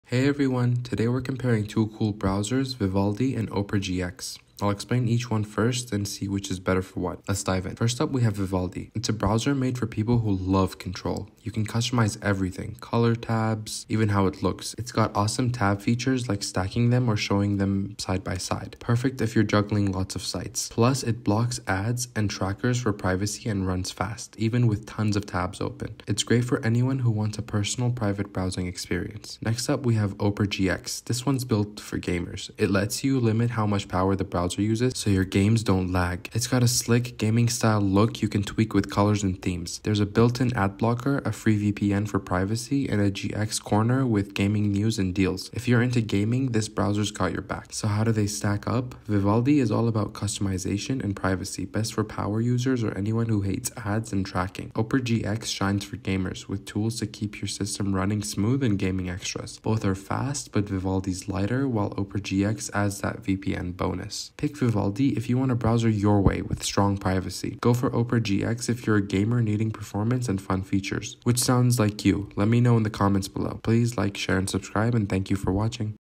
Vivaldi Browser vs Opera GX – Which Browser is MOST SECURE in 2025? (FULL REVIEW!)
In this video, we dive deep into the features and functionalities of two powerful web browsers: Vivaldi and Opera GX. Discover how Vivaldi's customizable interface stacks up against Opera GX's gaming-focused features. We'll compare speed, privacy options, user experience, and resource management, helping you decide which browser is best for your needs in 2025. Join us as we explore the pros and cons of each, highlighting their unique strengths and potential drawbacks. Don't miss out on this comprehensive comparison to find your perfect browsing companion! -